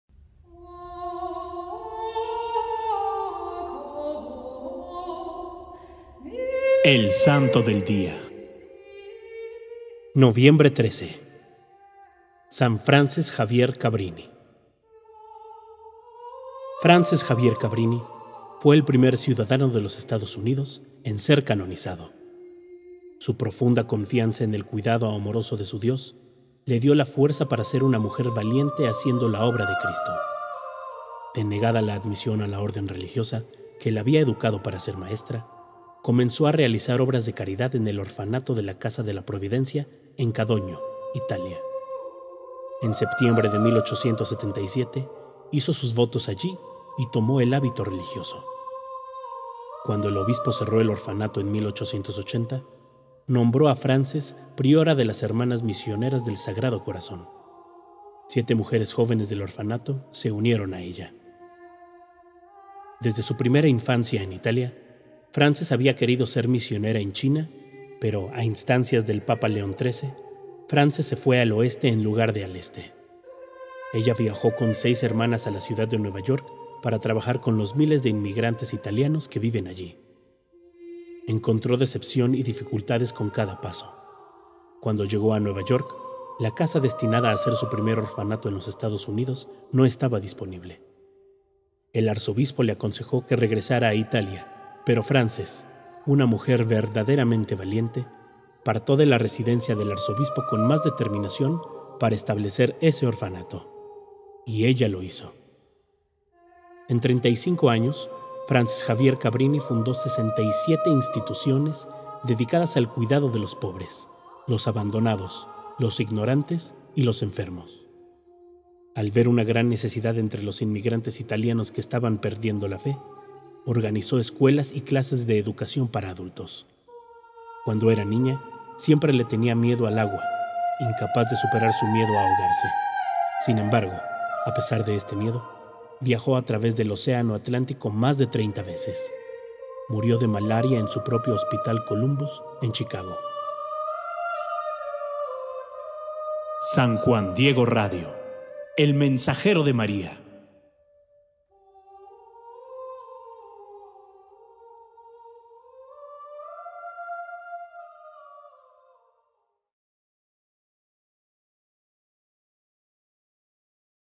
Santa Frances Xavier Cabrini
SANTO DE HOY, MARTES NOVIEMBRE 12
San Frances Xavier Cabrini
Frances Xavier Cabrini fue el primer ciudadano de los Estados Unidos en ser canonizado. Su profunda confianza en el cuidado amoroso de su Dios le dio la fuerza para ser una mujer valiente haciendo la obra de Cristo.

Denegada la admisión a la orden religiosa que la había educado para ser maestra, comenzó a realizar obras de caridad en el Orfanato de la Casa de la Providencia en Cadogno, Italia. En septiembre de 1877, hizo sus votos allí y tomó el hábito religioso.

Cuando el obispo cerró el orfanato en 1880, nombró a Frances priora de las Hermanas Misioneras del Sagrado Corazón. Siete mujeres jóvenes del orfanato se unieron a ella.

Desde su primera infancia en Italia, Frances había querido ser misionera en China pero, a instancias del Papa León XIII, Frances se fue al oeste en lugar de al este. Ella viajó con seis hermanas a la ciudad de Nueva York para trabajar con los miles de inmigrantes italianos que viven allí.

Encontró decepción y dificultades con cada paso. Cuando llegó a Nueva York, la casa destinada a ser su primer orfanato en los Estados Unidos no estaba disponible. El arzobispo le aconsejó que regresara a Italia. Pero Frances, una mujer verdaderamente valiente, partió de la residencia del arzobispo con más determinación para establecer ese orfanato. Y ella lo hizo.

En 35 años, Frances Xavier Cabrini fundó 67 instituciones dedicadas al cuidado de los pobres, los abandonados, los ignorantes y los enfermos. Al ver una gran necesidad entre los inmigrantes italianos que estaban perdiendo la fe, organizó escuelas y clases de educación para adultos.

Cuando era niña, siempre le tenía miedo al agua, incapaz de superar su miedo a ahogarse. Sin embargo, a pesar de este miedo, viajó a través del Océano Atlántico más de 30 veces. Murió de malaria en su propio Hospital Columbus en Chicago.
FUENTE:
TRADUCIDO POR GOOGLE TRANSLATOR